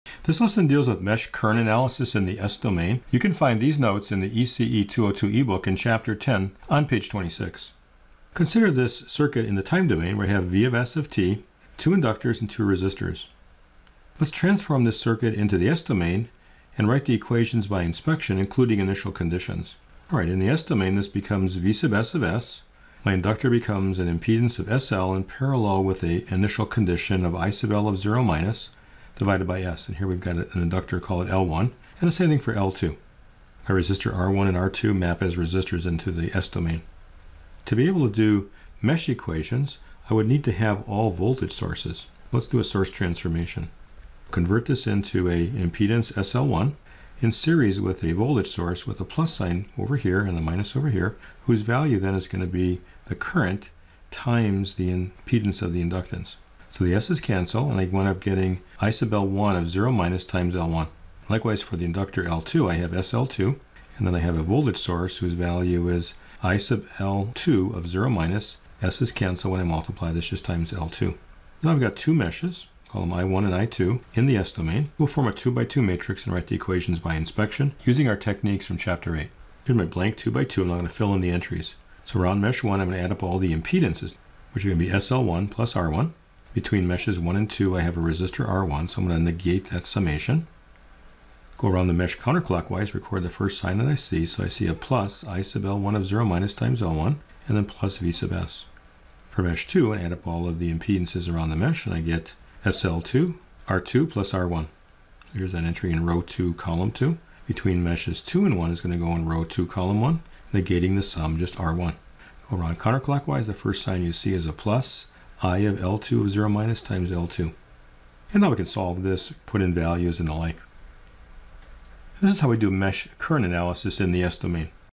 ECE202msu: Chapter 10 - Mesh-Current Analysis in the s-Domain
This video is a lecture from the ECE 202 ebook by Gregory M. Wierzba. The material covered is from Chapter 10 p 26.